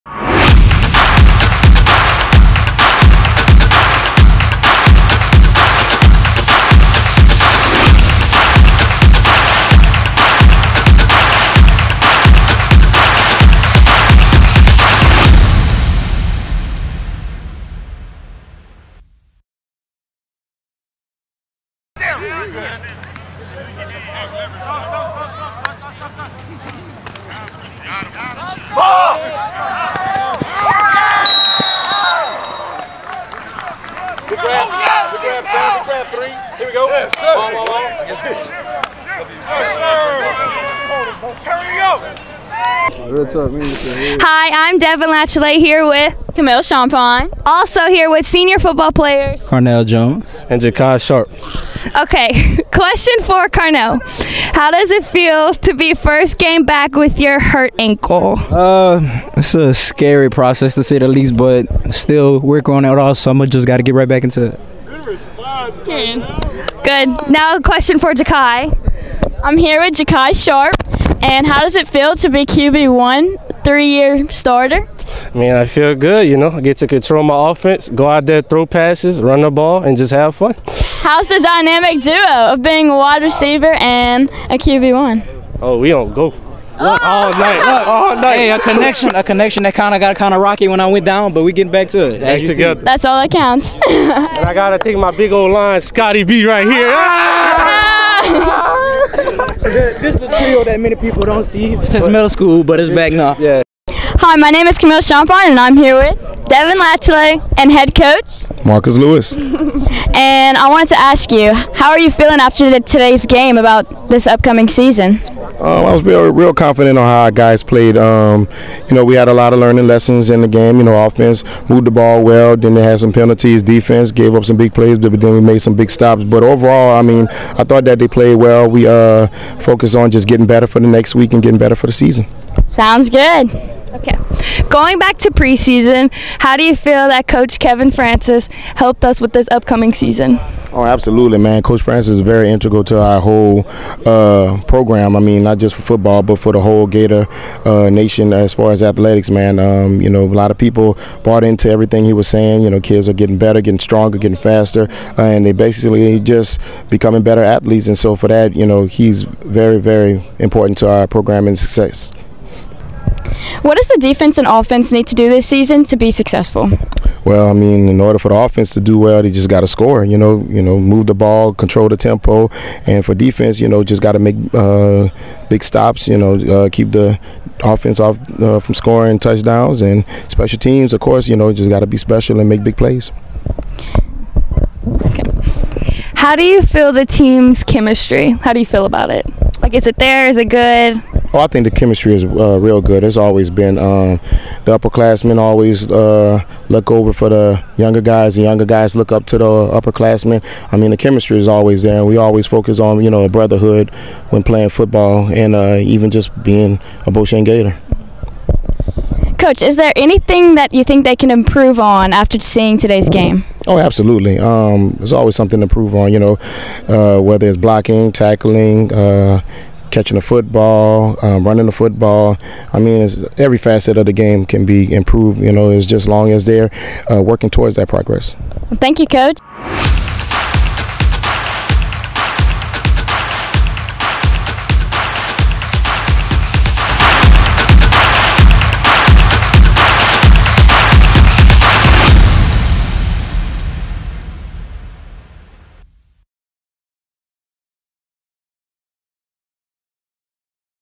2022 Beau Chene Football Report (Episode 301, post OC scrimmage)
Post OC Scrimmage interviews for Beau Chene High School Football Team.

Beau Chene Football Report
Hosted by Devyn Latiolais and Camille Champagne